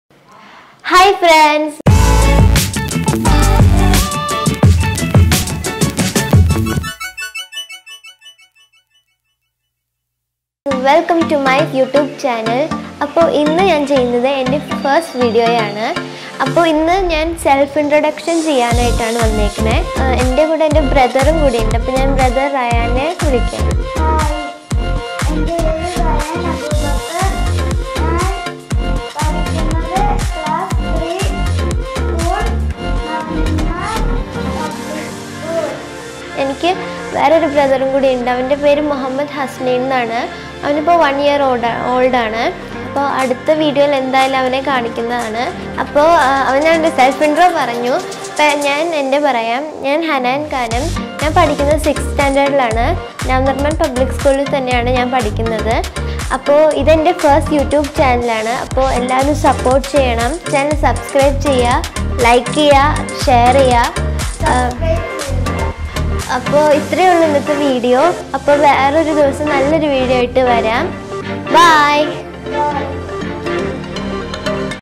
MY FIRST VLOG │ INTRODUCING MYSELF AND MY BROTHER
Hello viewers,
     I am Hanaan Khanam, from Kochi, Kerala. Hanaan's Vlog is my YouTube channel and  this is my first video on it. In this video i am introducing myself and my brother. In coming day i will upload many interesting videos on lifestyle and many more. Please give your blessings and support me.